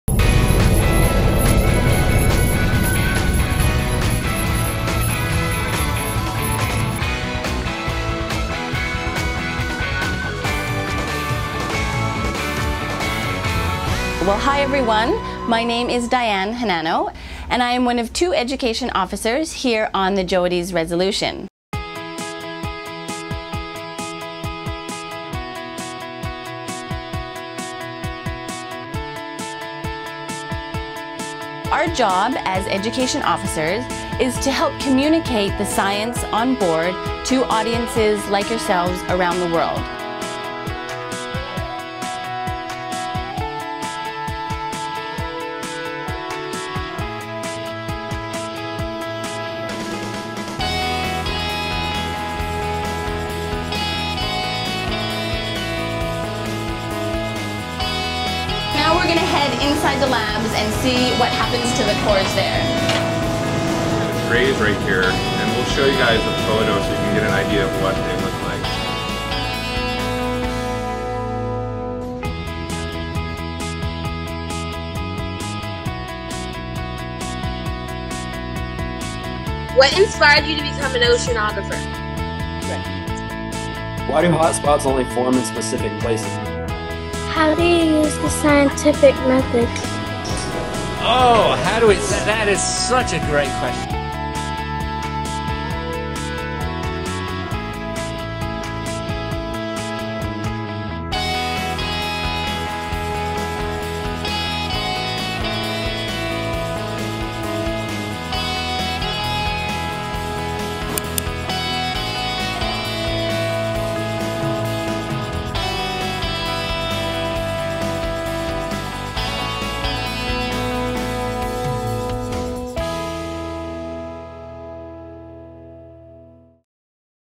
JR Live!
Want to know what it's like to participate in a live ship-to-shore event from the JOIDES Resolution? Watch here to find out!